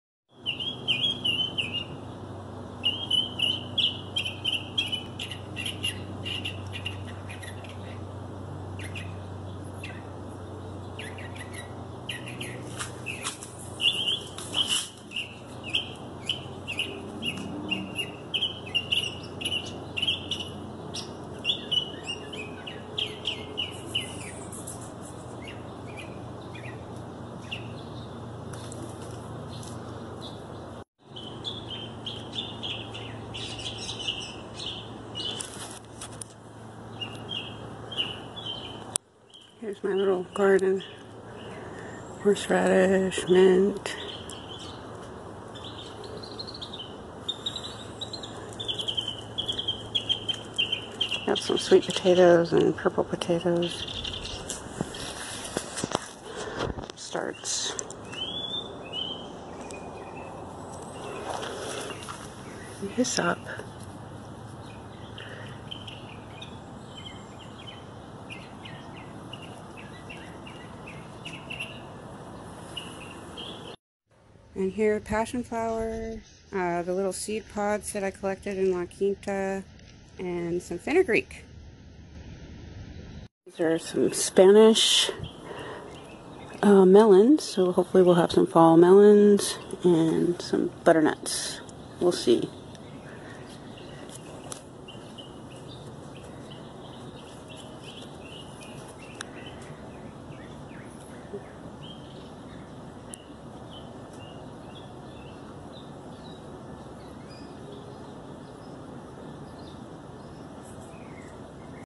Birdsong ? of Identity & Fall Garden Experment
I heard this song and had to record the beautiful sounds. I am not sure and haven't had a chance to research what kind of bird this is.
Garden plants partial list:
Mandevilla Vine harvest from La Quinta